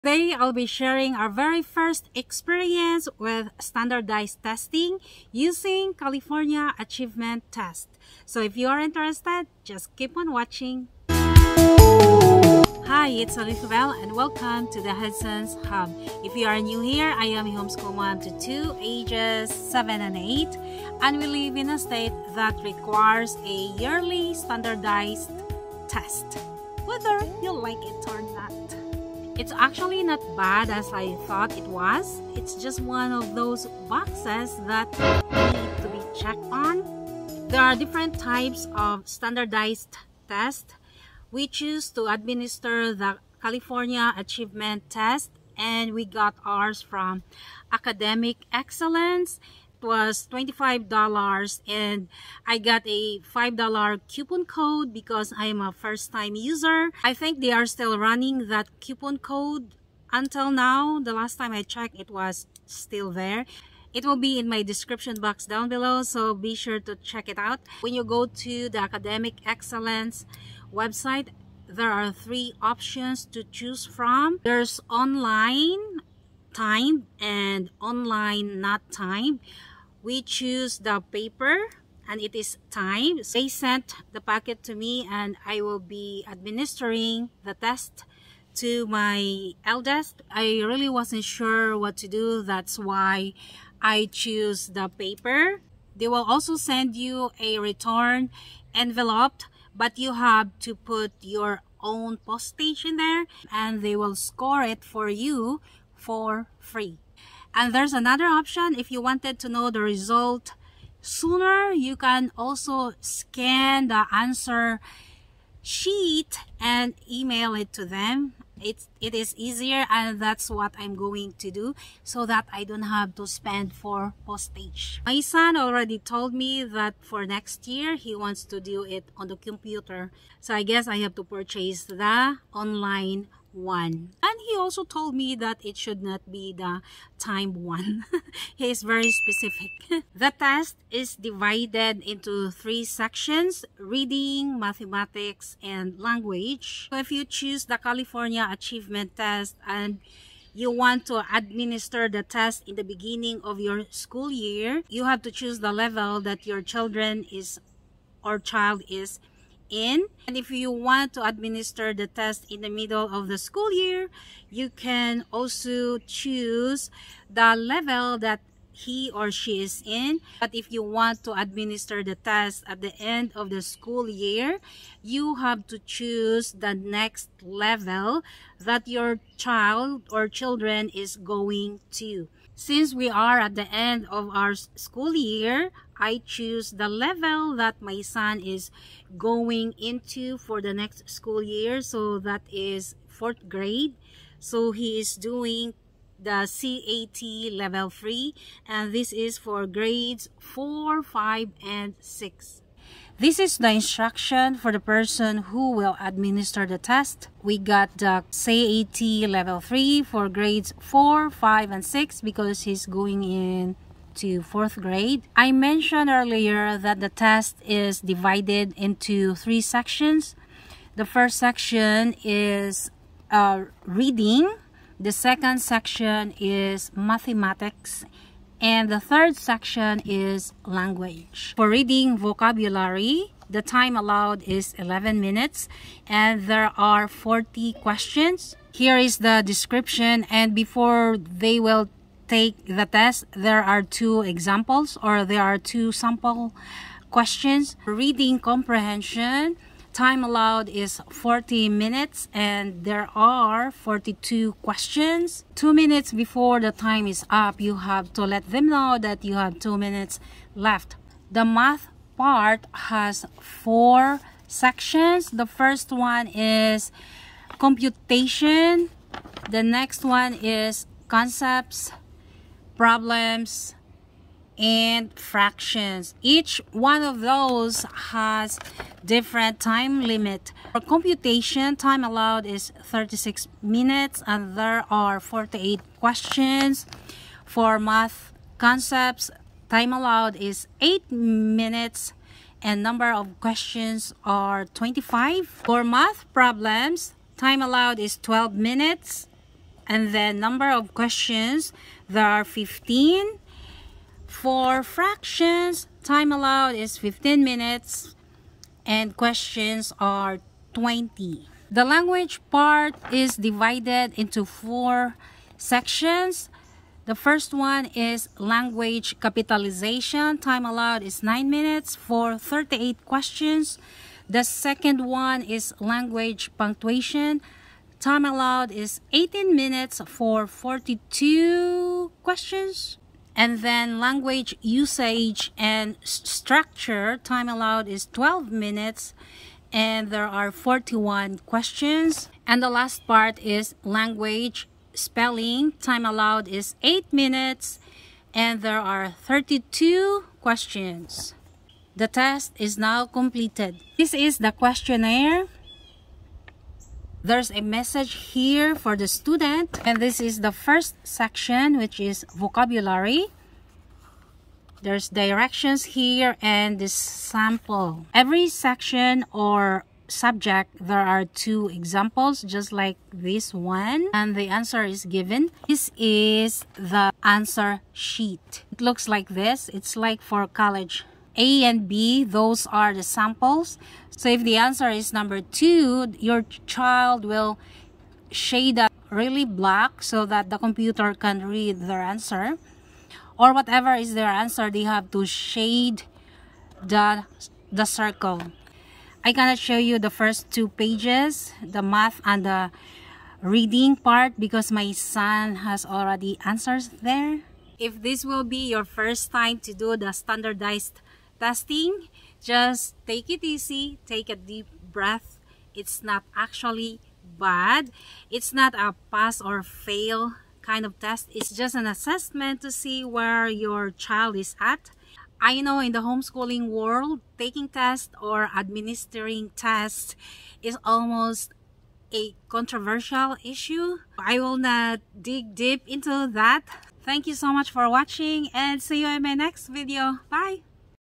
Homeschool Standardized Testing | California Achievement Test
In this video, you are going to see an overview of the California Achievement Test packet.

Find the CAT Test packet here👇
$5 off (min. order $10) on Your First Purchase